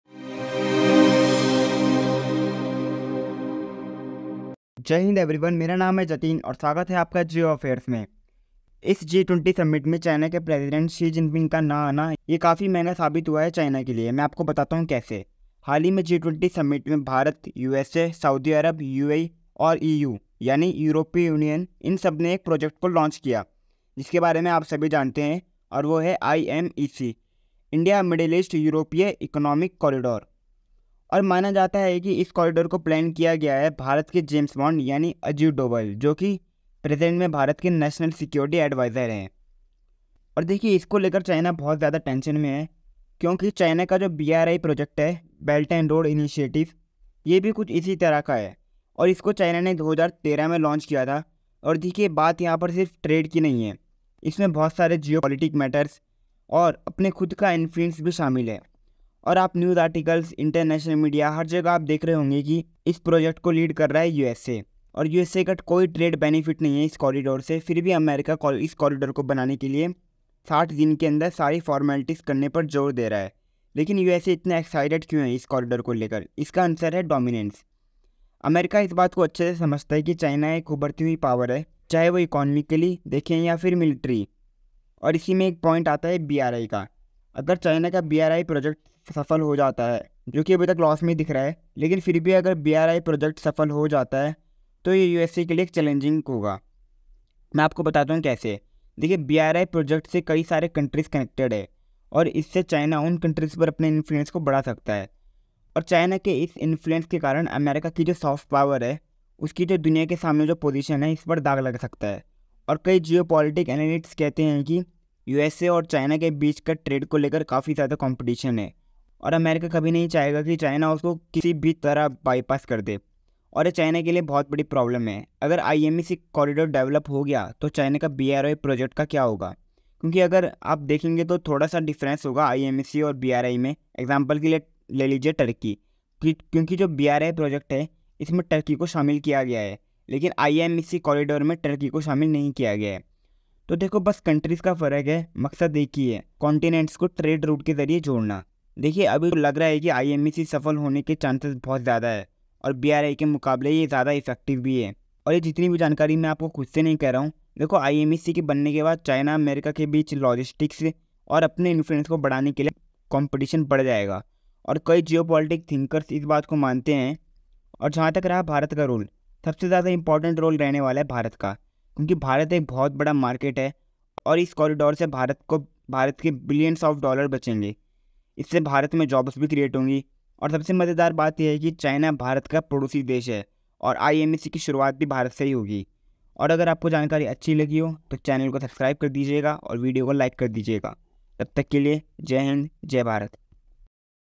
RIVALRY BETWEEN "IMEC CORRIDOR" AND "BRI PROJECT" | चीन की बड़ी मुश्किलें | By Geo affairs
RIVALRY BETWEEN"IMEC CORRIDOR" AND "BRI PROJECT" | चीन की बड़ी मुश्किलें ! Exaplin in hindi| By Geo affairs

Jai hind everyone.. haal hi main  G20 summit ke doran bharat,USA,UAE,SAUDI ARAB, or EUEROPEON UNION ke bich IMEC corridor project sign hua hai, or isko Lekr BRI PROJECT pr ky asar padega, isko maine explain Kiya hai agr apko jankari axi lge to channel ko subscribe kr dijiyega or video ko like kr dijiyaga,
JAI HIDJ JAI BHARAT ❤️🇮🇳